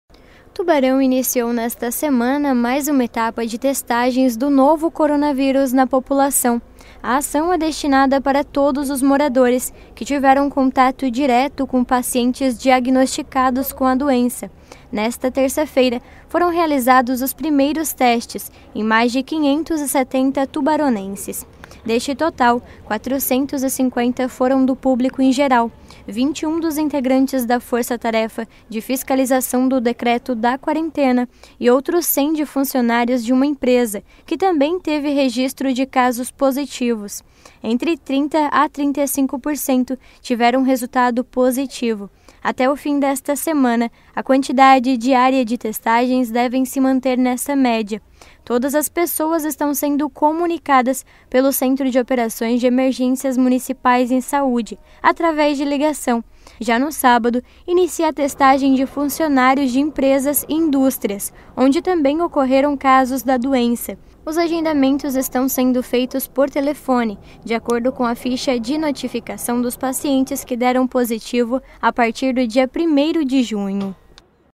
Testes corona.
Nesta terça-feira (21/07) iniciou aqui em Tubarão, mais uma etapa de testagem para detecção do novo Coronavírus no município. Neste primeiro dia, mais de 570 tubaronenses foram testados.